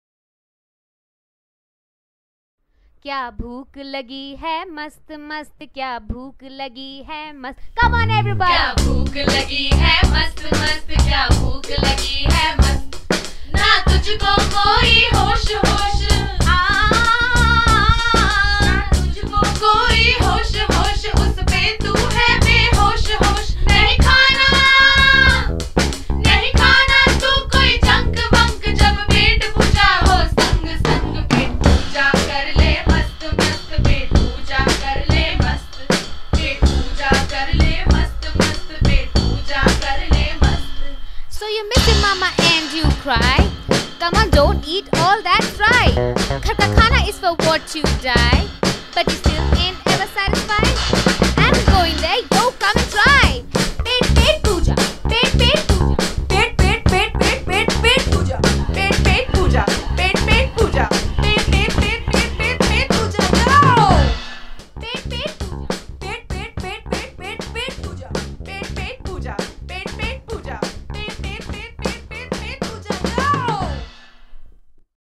Pett Pooja jingle
The glam girls of Pett Pooja selling... Fun jingle as part of their assignment...